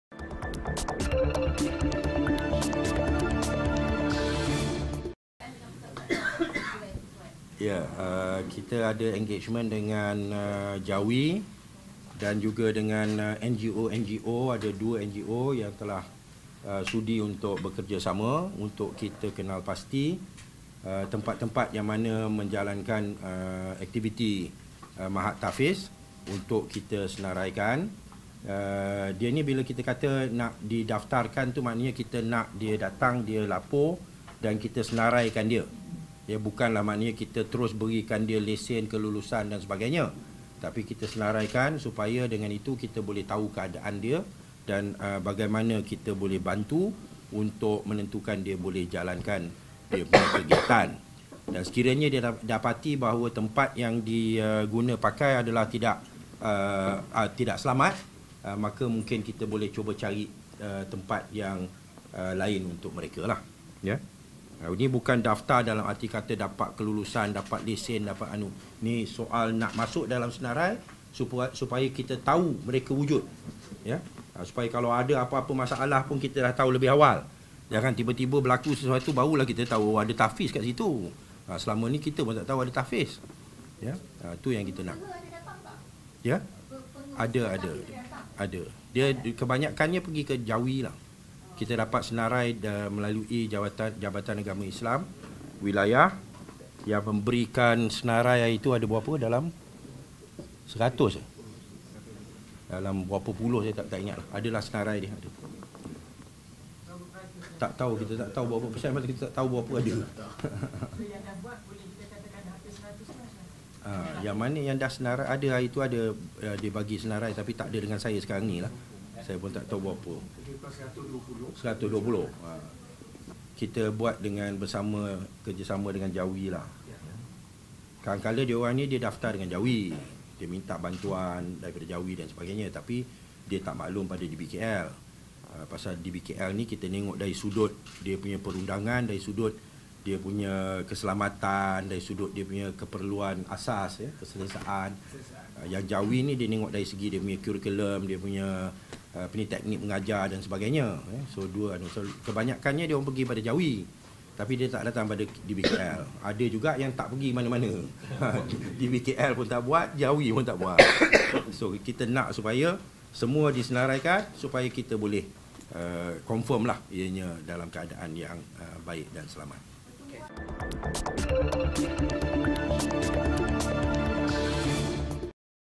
Khalid: Permohonan lesen oleh pusat tahfiz ‘haram’ tidak semestinya diluluskan
Sekolah atau pusat tahfiz yang beroperasi secara haram dan membuat permohonan pendaftaran lesen kepada Dewan Bandaraya Kuala Lumpur (DBKL) dan Jabatan Agama Islam Wilayah Persekutuan (Jawi) tidak semestinya diluluskan.

Menteri Wilayah Persekutuan, Khalid Abdul Samad berkata, pihaknya akan membuat tapisan serta pemantauan sama ada kawasan operasi pusat tahfiz berkenaan sesuai atau sebaliknya terlebih dahulu sebelum meluluskannya.